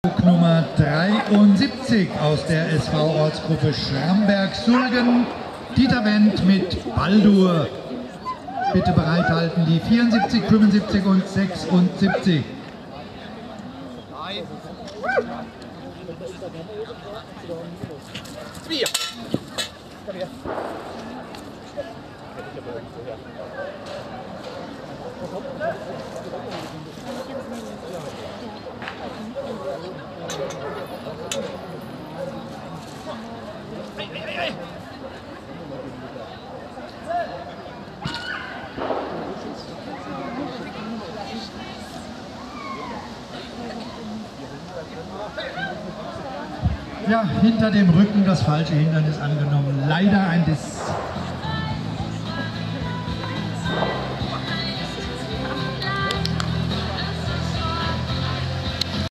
Dieter Wendt & Baldur A 3 - SV Meisterschaft am 06.10.2012 in Darmstadt
Richter: Inge Kurz, Sollzeit 47 s, Länge 164,50 m, 3,50 m/s - 73 Starter
Dieter & Baldur  A 3 - SV OG Schramberg-Sulgen